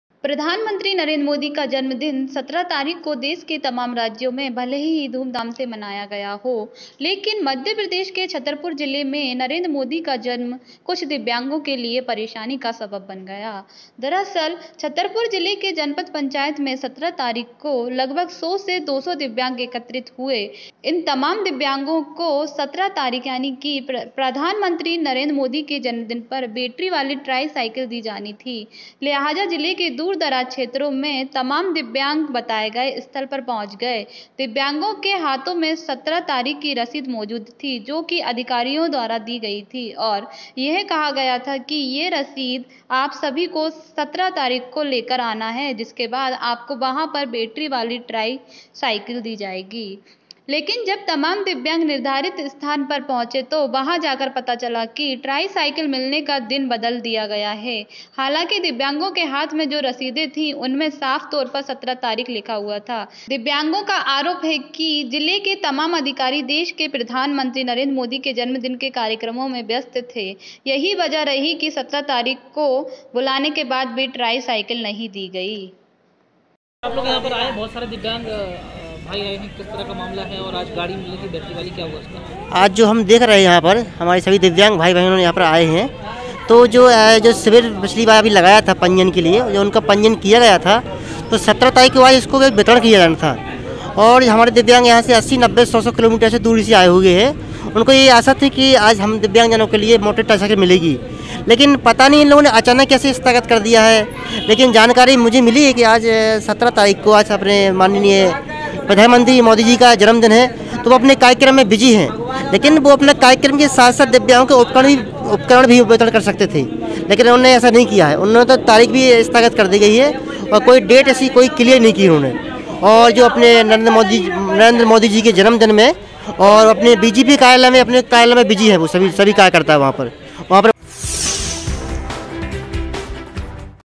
मोदी के जन्मदिन पर दिव्यांगों से मजाक । TIME8 MADHYA PRADESH
छतरपुर। जिले में नरेंद्र मोदी का जन्मदिन पर कुछ दिव्यांगों को कई समस्‍याओं का सामना करना पड़ा। जनपद पंचायत में बैटरी बाली ट्राई साइकिल के लिये 17 तारीख को लगभग 100 से 200 दिव्यांग एकत्रित हुई, लेकिन उन्हें साइकिल उपलब्‍ध नहीं कराई गई।